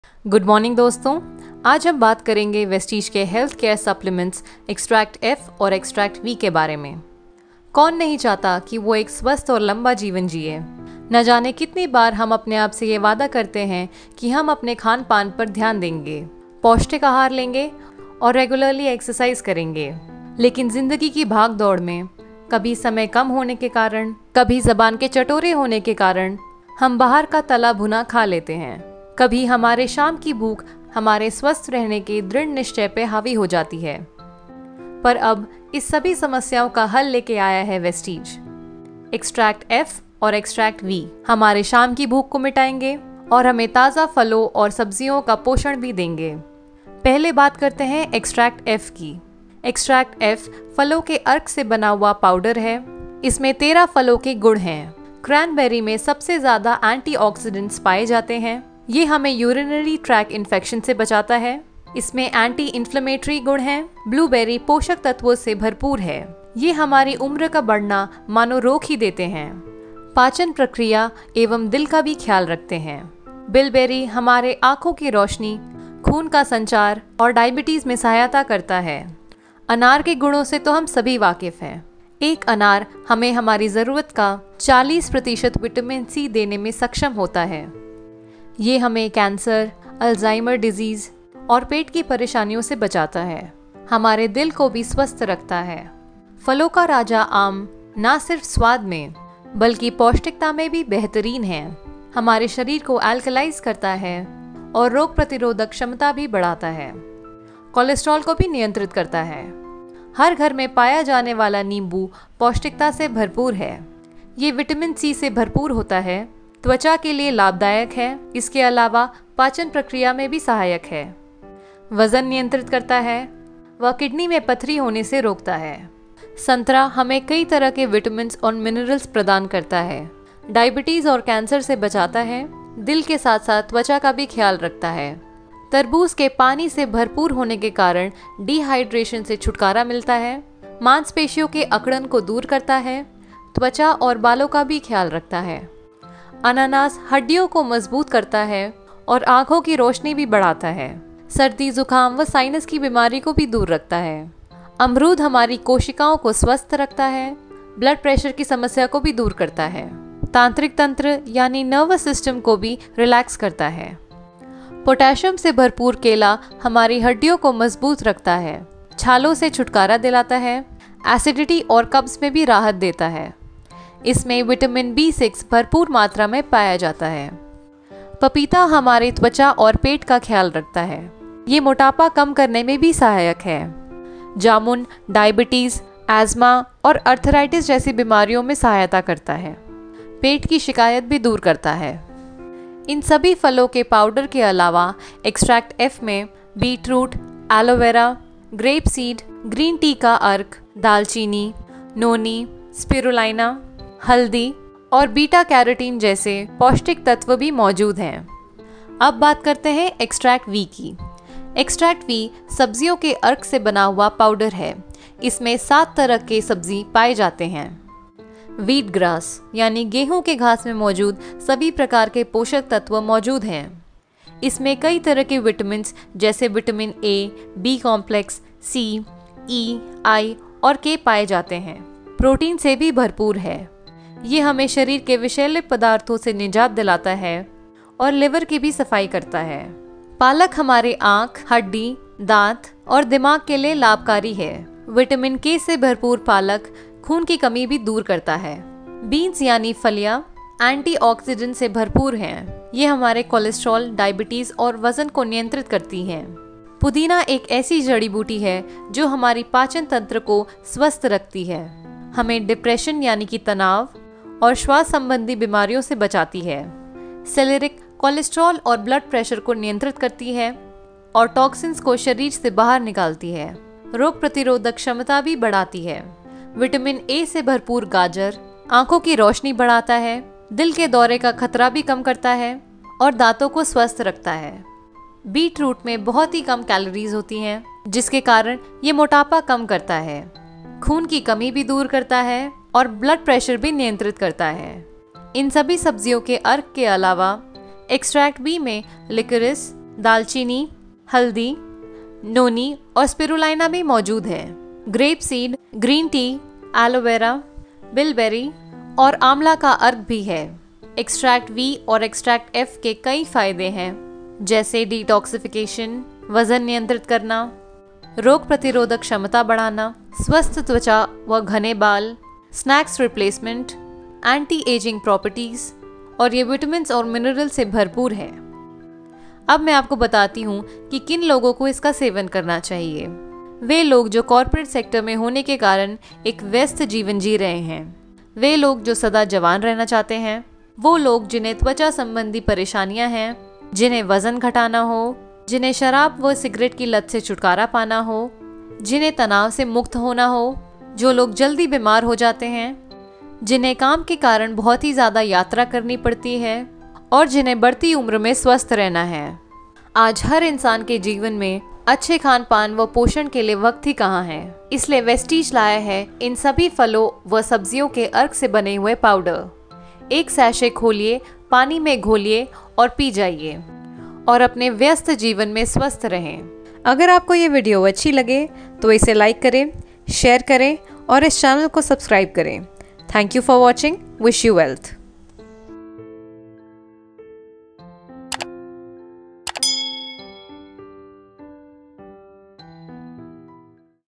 Vestige Prime X-Tract F & X-Tract V (in Hindi)
Dosage: One sachet in a glass of water everyday.

🌿VESTIGE PRIME X-TRACT V:

CONTAINS 7 VEGETABLES WHEAT GRASS, SPINACH, BEANS, MINT, CELERY, CARROT, BEETROOT In addition to vegetable powders, Vestige Prime X-Tract V also contains nutritious additives like – licorice, cinnamon, turmeric, spirulina and noni. It also contains extracts of Green Tea, Grape Seed, Bilberry, Amla, and Aloe Vera. It contains added phytochemicals like Beta Carotene and Quercetin known for their antioxidant properties. Vestige Prime X-Tract V helps to fulfil the daily requirement of essential vitamins and dark green leafy vegetables; it is full of phytochemicals, chlorophyll and fibre that keep you full for longer period The added extracts give an extra boost of essential nutrients to your body keeping you nourished and active all day long.

🍉 VESTIGE PRIME X-TRACT F:

Contains 13 fruits BEET ROOT, CRANBERRY, BLUEBERRY, BILBERRY, POMEGRANATE, MANGO, LEMON, ORANGE, WATER MELON, PINEAPPLE, GUAVA, BANANA, PAPAYA, BLACK PLUM In addition to fruit powders, Vestige Prime X-Tract F contains nutritious additives like – cinnamon, turmeric, spirulina and noni. It also contains, Aloe vera, Alfalfa and Beta Carotene with high antioxidant value. Vestige Prime X-Tract F helps to fulfil the daily requirement of essential nutrients and colourful fruits ranging from everything you need in your daily diet. The added extracts give an extra boost of antioxidants and essential nutrients to your body, making you healthy and strong. It is full of antioxidants and fibres that help to fight free radicals and keep you full for a longer period.